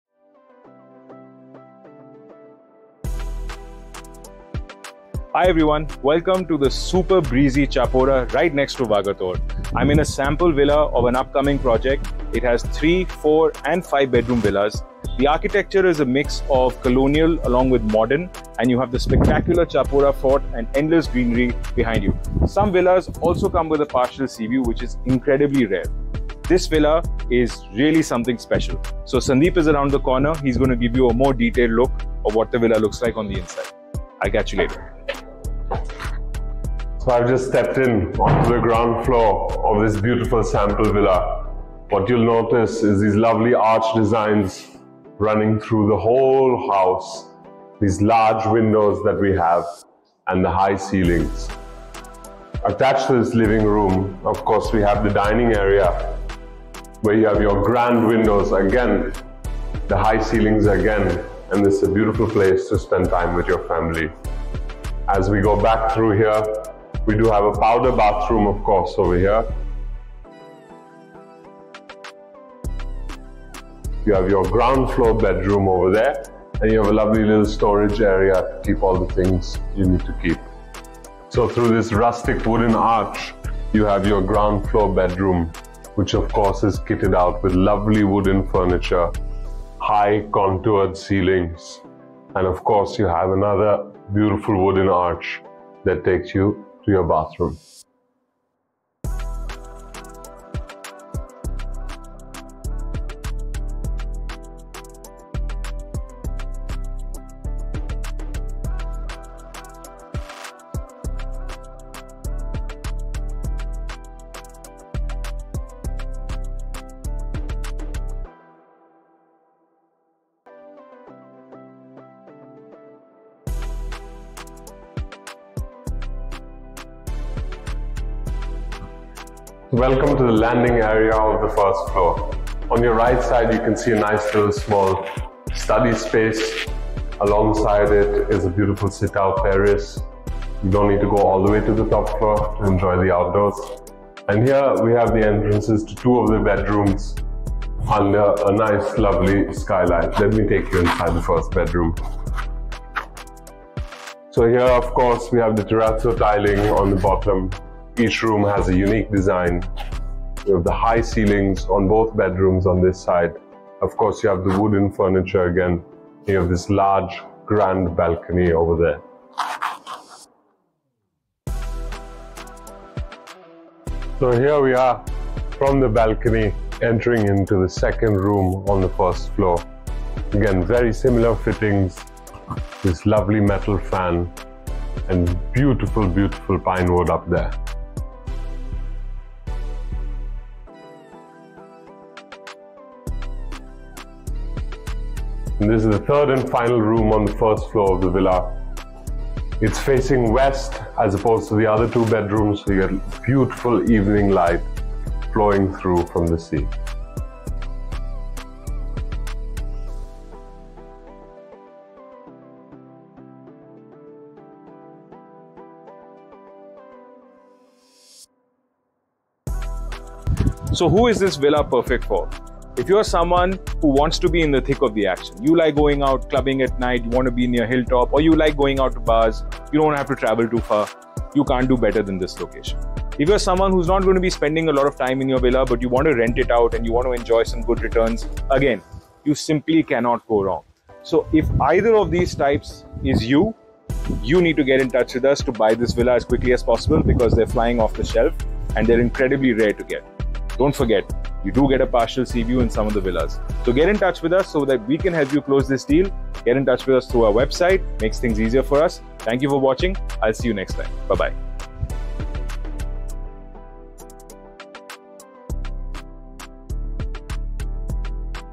Walkthrough of newly built Luxury Villa in Vagator Goa | Tpb
Luxury villas in Vagator with Seaviews! Choose between 3, 4 and 5 bhk villas!

Tpb is a boutique brokerage firm featuring Luxury Homes in leisure destinations!
Luxury Homes For Sale In Goa, Greece & Cyprus!